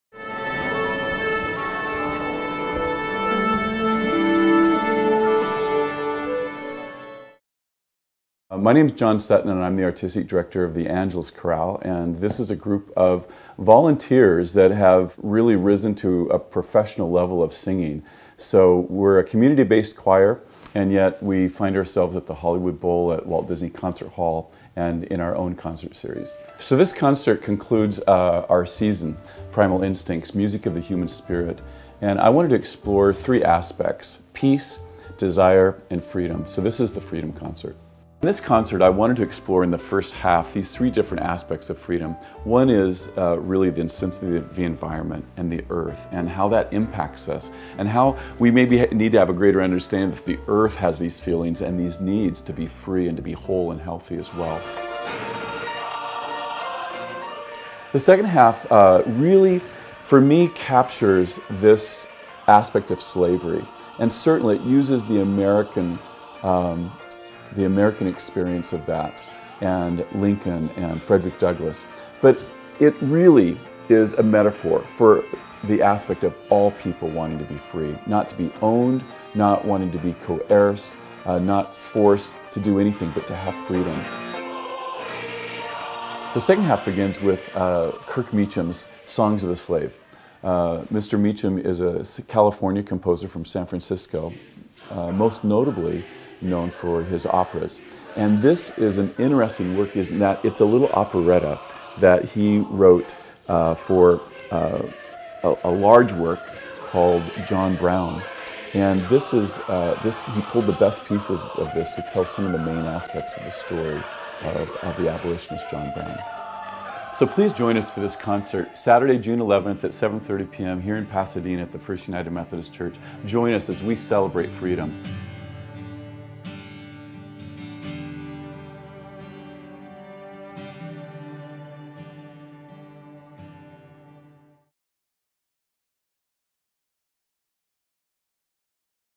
John Sutton talks about the "FREEDOM" Concert, June 11, 2016
Angeles Chorale Director John Sutton explains the exciting program of music that will be featured at "FREEDOM: The Sounds of Hope and Survival", the June 2016 concert.